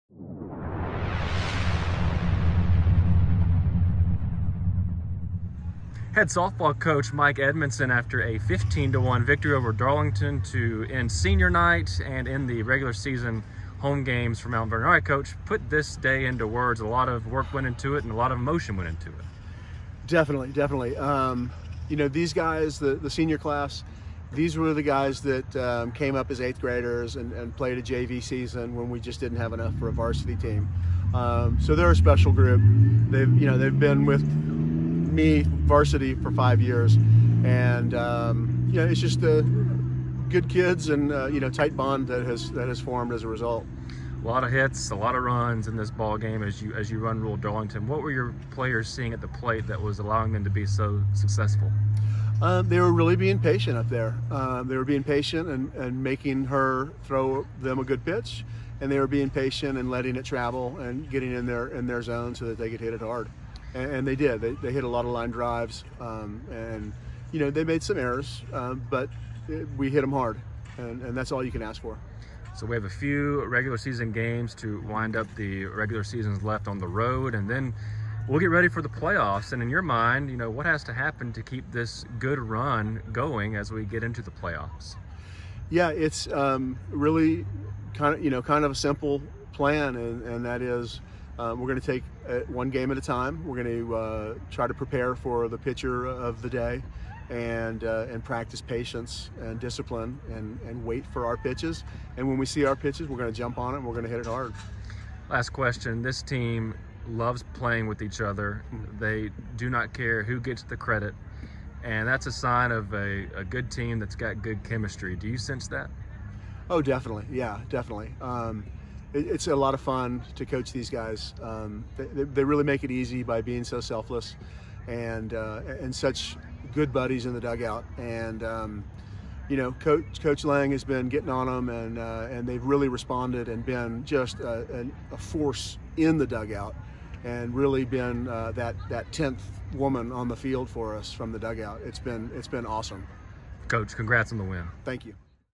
Softball Recap - Darlington - Head Coach Mike Edmonson - 9-28-21
Head Coach Mike Edmonson comments on Mount Vernon's 15-1 victory over Darlington on Senior Night.